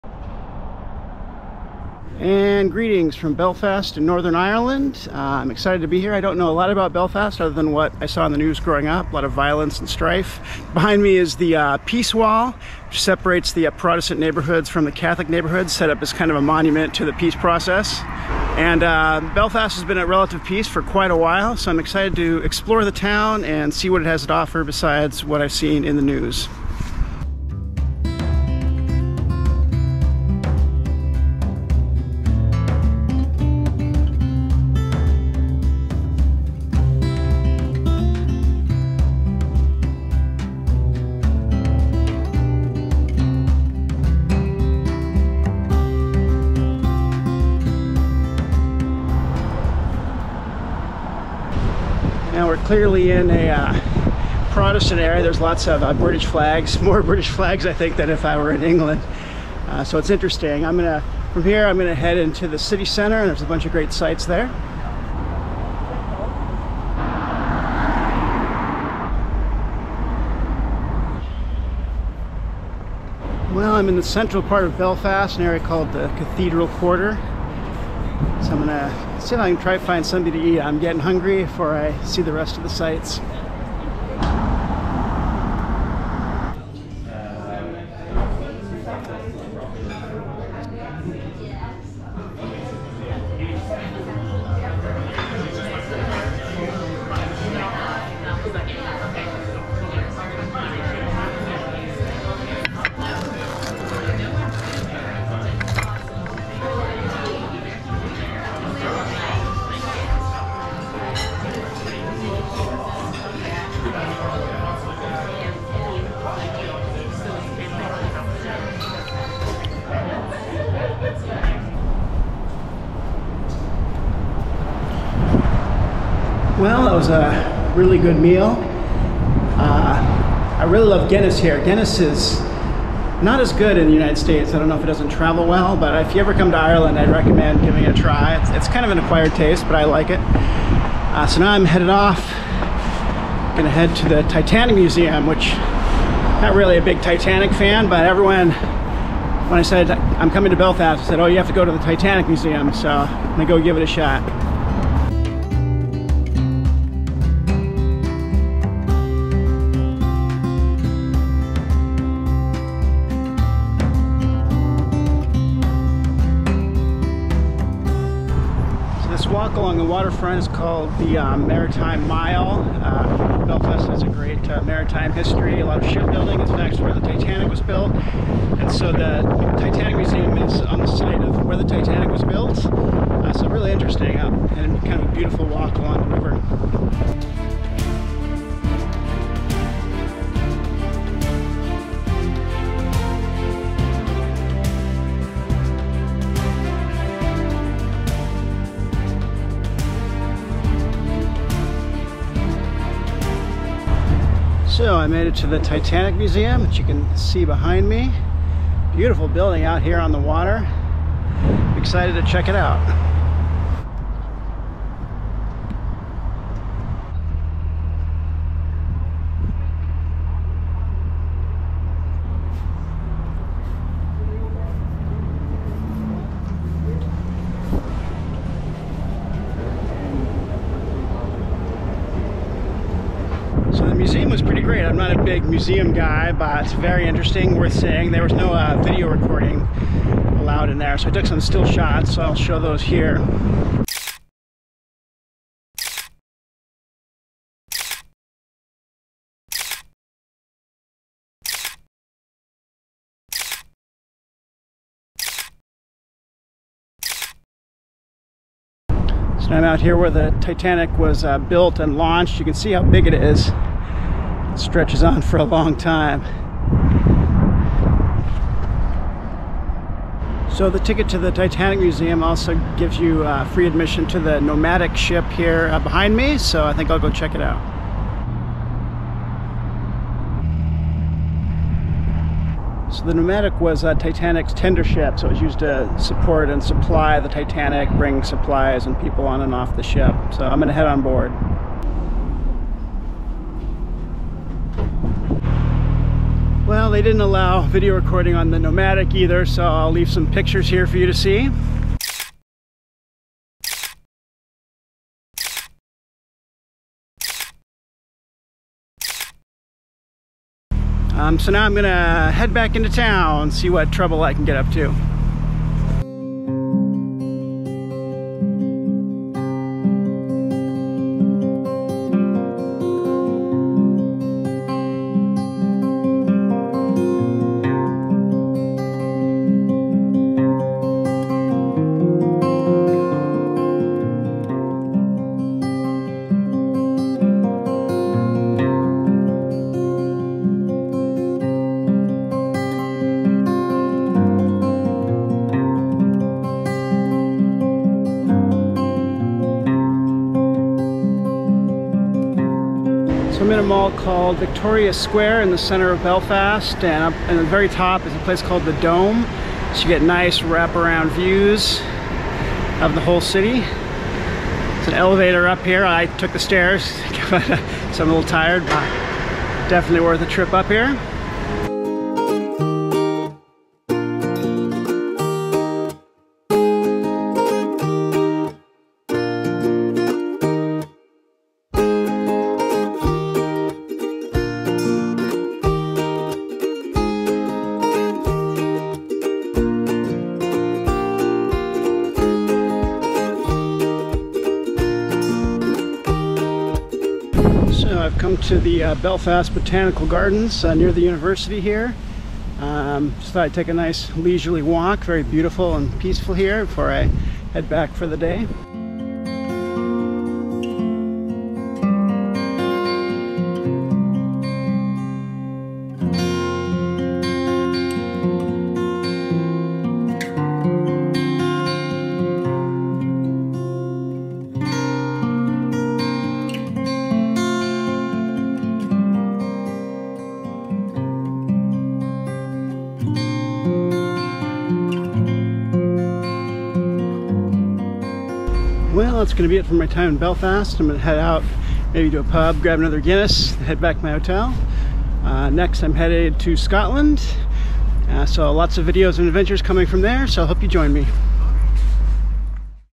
One Day in Belfast, Northern Ireland.
I visit the city of Belfast to see how it has adapted from its once dangerous reputation. I tour the Titanic Museum, stroll through the beautiful Botanical Gardens, and even enjoy a pint of Guinness.